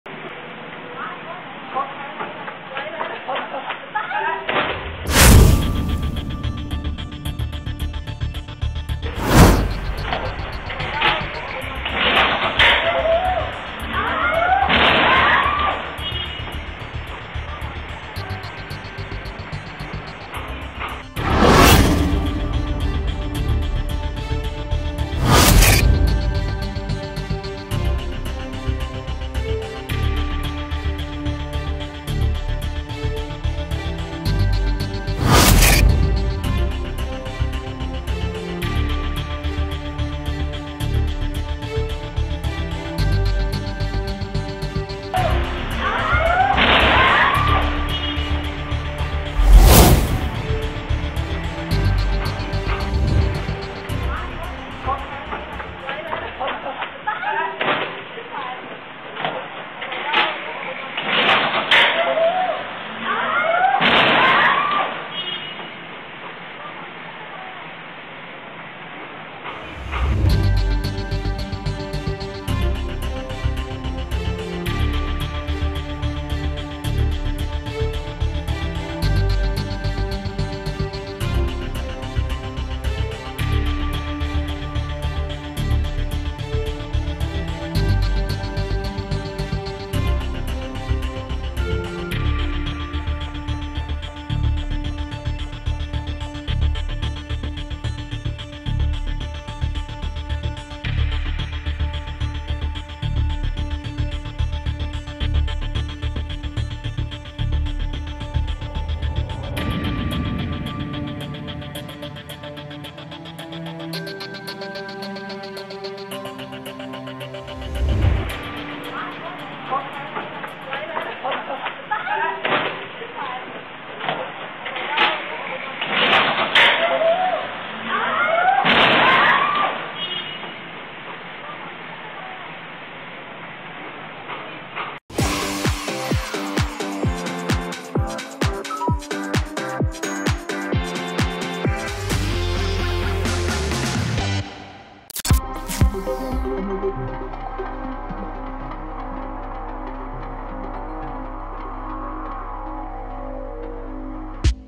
มือใหม่หรือเปล่า พยายามหยุดรถไหลแต่พุ่งออกถนน
อุทาหรณ์ผู้ขับขี่! อยู่ดีๆ รถไหลพุ่งจะออกถนน โชคดีมีเสากั้นสกัดเหตุบานปลาย คาดพยายามจะเบรคแต่ดันเหยียบคันเร่ง